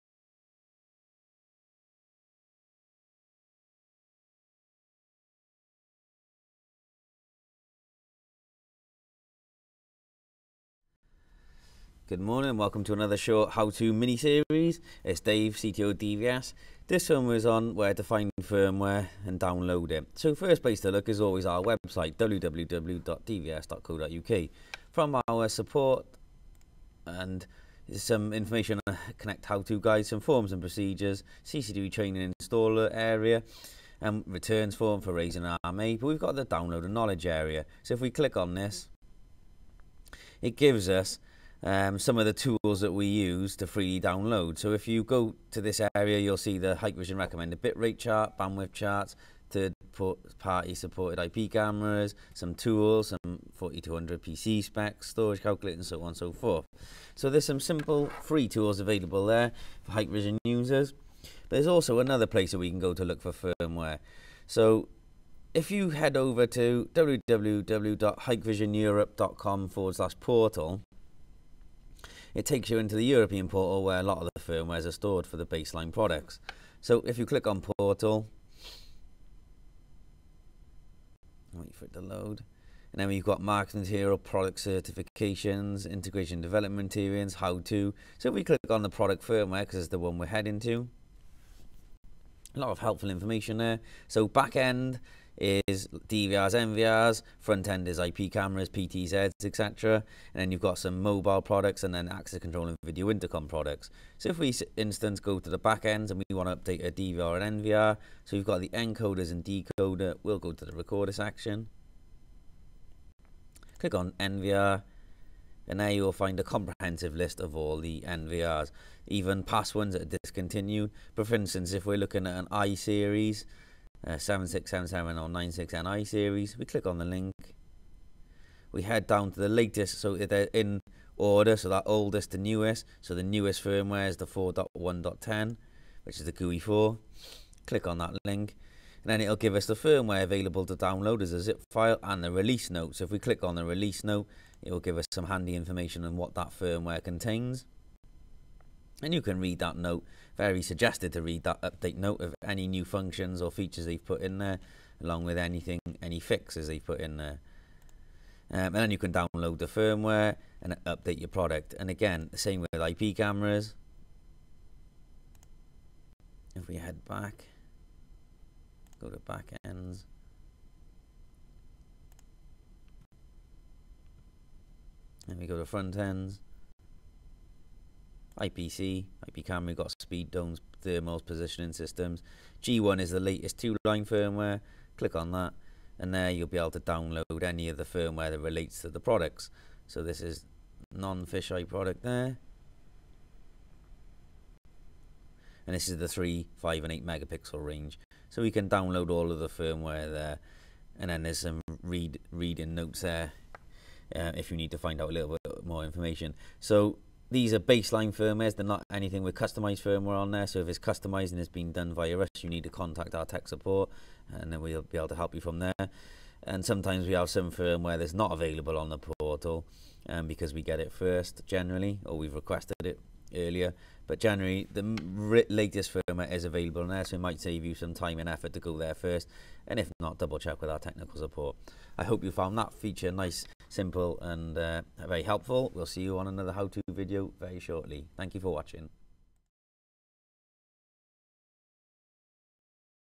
Where to download the latest Hikvision Firmware
In this video we tell you where to find the latest Hikvision firmware.